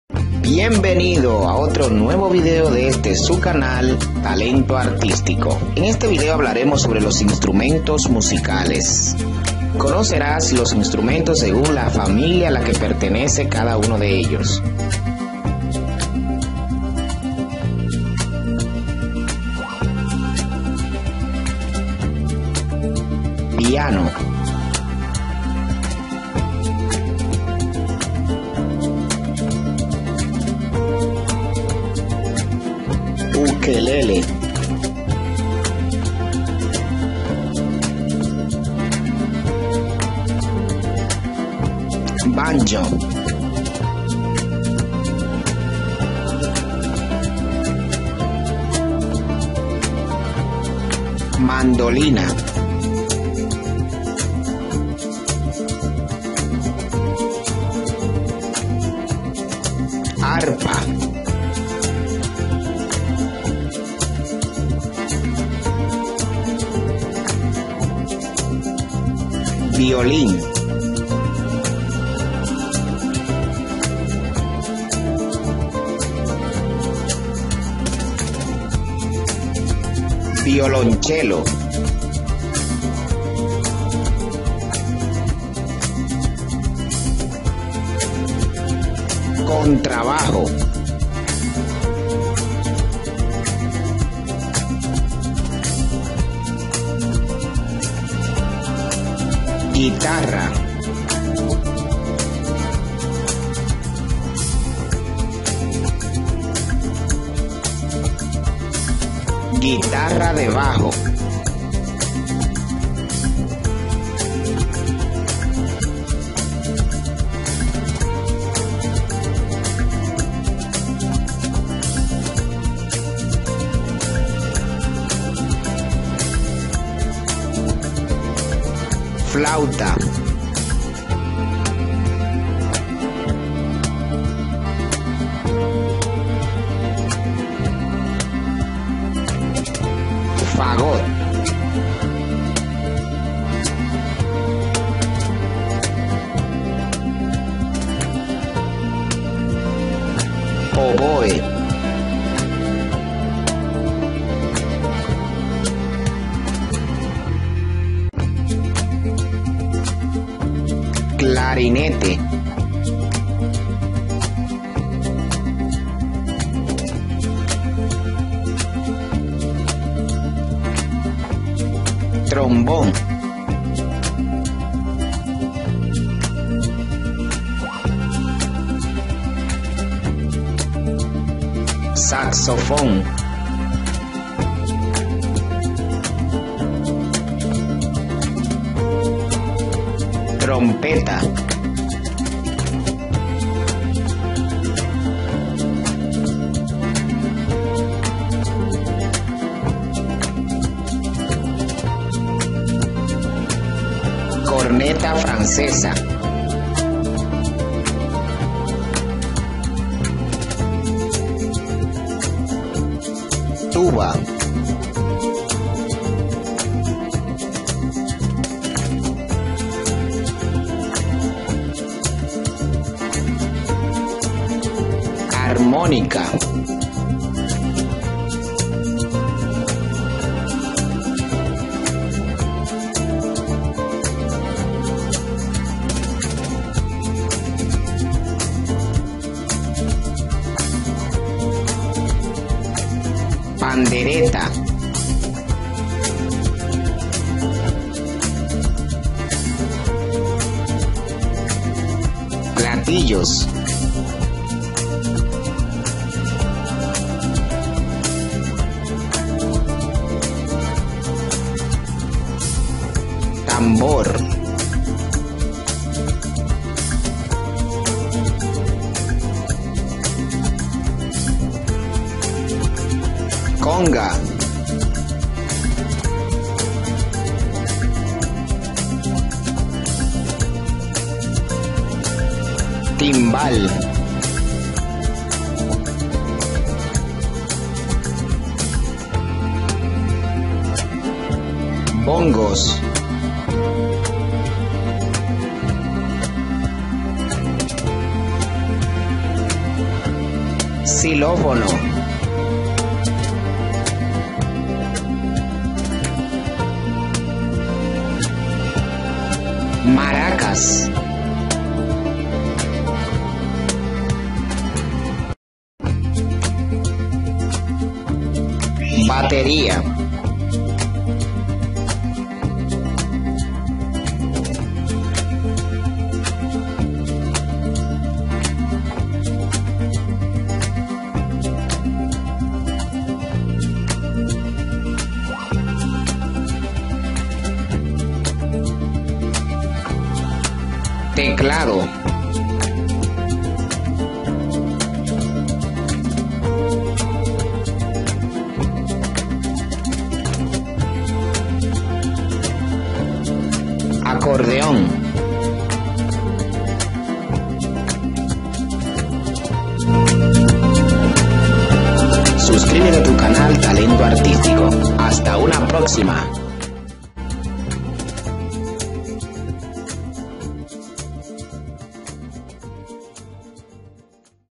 IDENTIFICA los INSTRUMENTOS musicales, según la familia a la pertenezca
Existen instrumentos que ayudan a marcar el ritmo como son los de percusión, o aquellos que producen el sonido al golpearse, frotarse o sacudirse como la tambora, la batería, la güira, la pandereta, las maracas, etc.

Algunos de los instrumentos de viento son la flauta, la trompeta, el trombón, el saxofón, el clarinete, la tuba, etc. Mientras que los instrumentos de cuerda son armónicos pues se pueden producir varios sonidos y diferentes acordes con un mismo instrumento como es el caso del piano, la guitarra, el arpa, el violín, el violonchelo, etc.

Recordemos que los instrumentos de cuerda producen el sonido al frotar sus cuerdas con los dedos como el arpa; Con una púa, como la guitarra; O con un arco, como es el caso del violín y el violonchelo.

Información básica sobre diversas manifestaciones del arte.